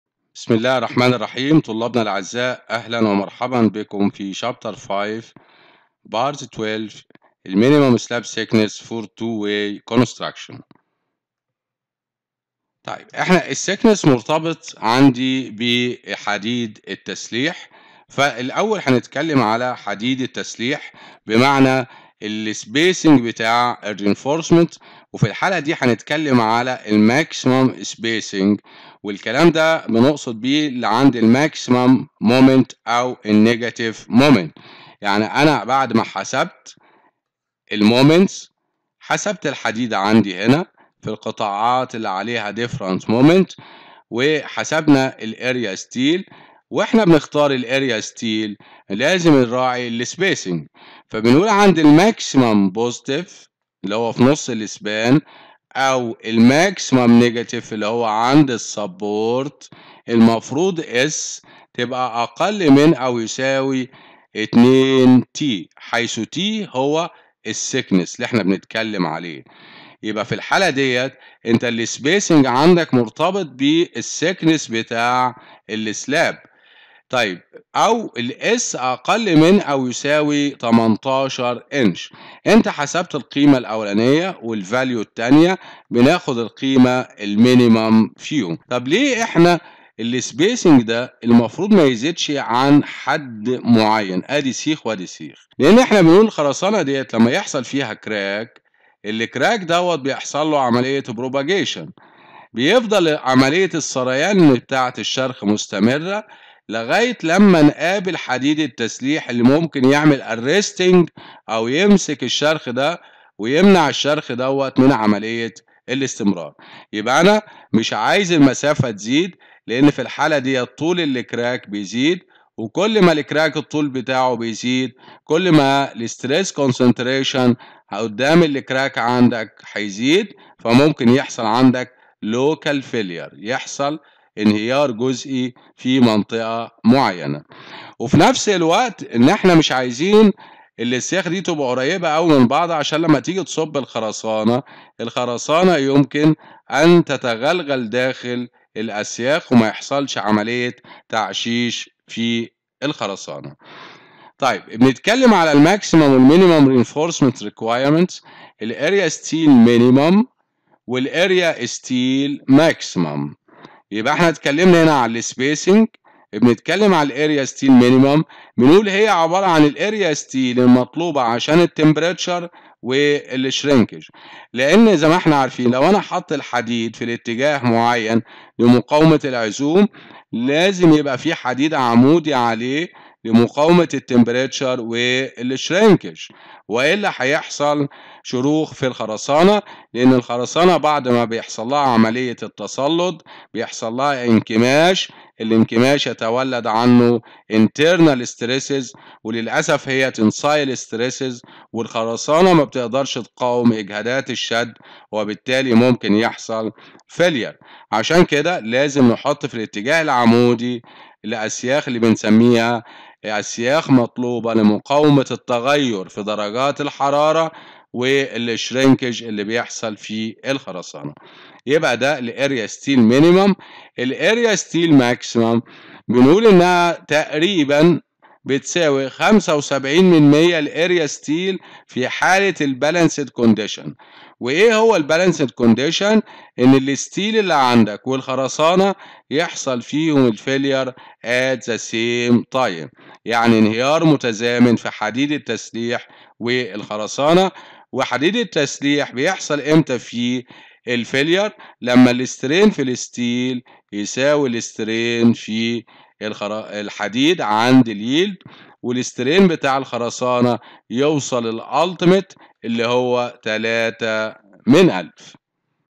Reinforced Concrete - 2 | C5-L12 | Minimum Slab Thickness for two-way construction
Chapter 5 : Design of Two-Way Floor Slab System
Lecture 12 : Minimum Slab Thickness for two-way construction